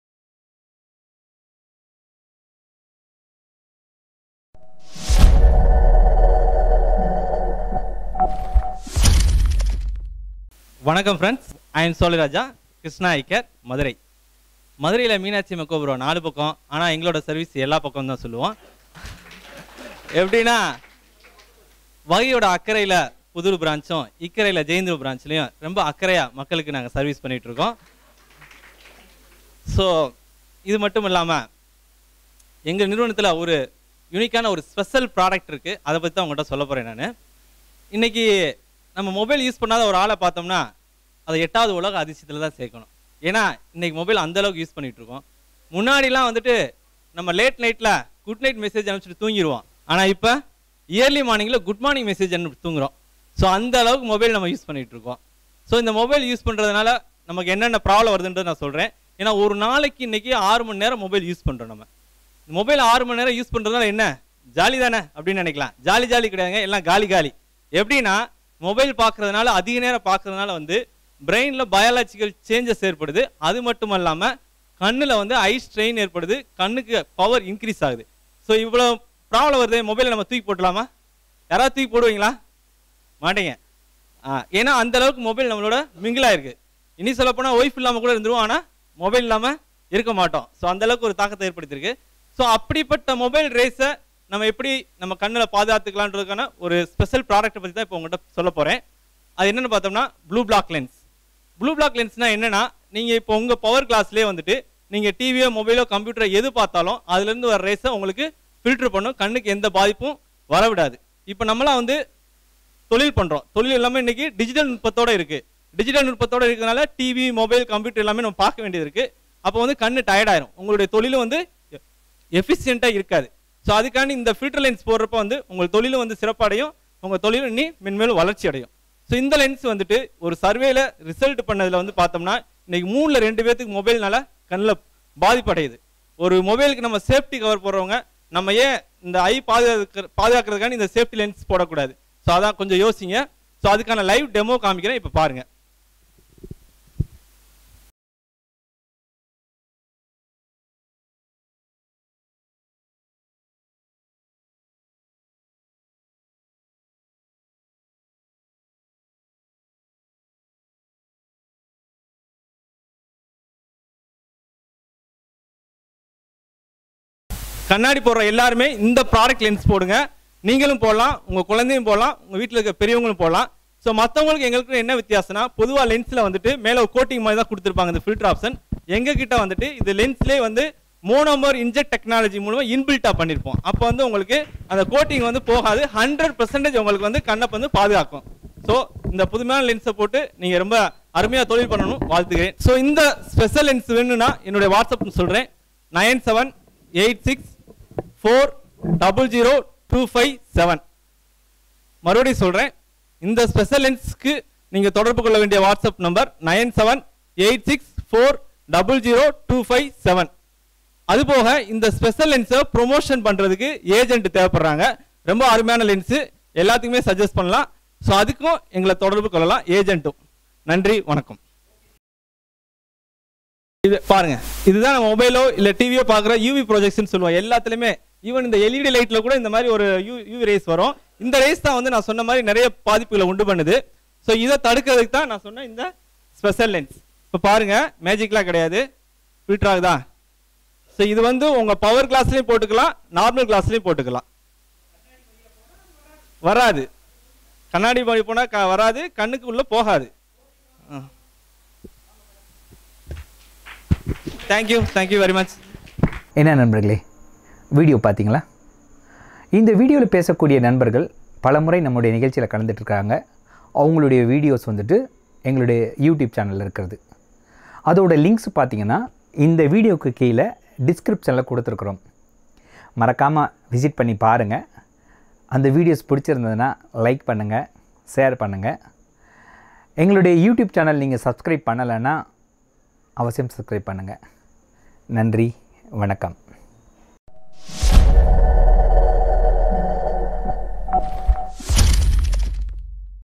அதிக நேரம் மொபைல் பார்ப்பவர் கண்ணுக்கு ஆபத்து! இந்த ஸ்பெஷல் கண்ணாடி அணிந்து பார்த்தல் ஆபத்தை...
அதிக நேரம் மொபைல் பார்ப்பவர் கண்ணுக்கு ஆபத்து
இந்த ஸ்பெஷல் கண்ணாடி அணிந்து பார்த்தல் ஆபத்தை தவிர்க்கலாம்

வியாபார வாய்ப்பும் உண்டு

M. Solairaja, Krishna Eye Care Opticals, Madurai ( Jaihindpuram & Pudhur)

krishnaeyecareopticals

Eye Testing & opticals
Businesspannalam com's 13th Business Meet @ Coimbatore - Nov 2, 2019